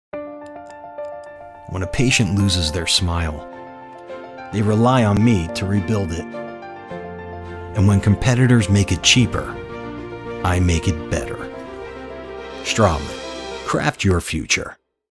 Straumann Group Digital Solutions - Shape Your Future
When a patient loses their smile, they rely on you to rebuild it. And when competitors make it cheaper, you make it better.

At Straumann Group, we have built a complete suite of hardware and software to help you work better. But at the end of the day, we are here to help you rebuild smiles for your patients.

We want to equip you with specially selected equipment, materials, software, and services tailored to the needs of your lab. We cover:

• Scanning
• Planning and Designing
• Milling
• Printing
• Prosthetic Solutions

All these solutions come with the shared expertise of our world-class support team. Ready to learn more?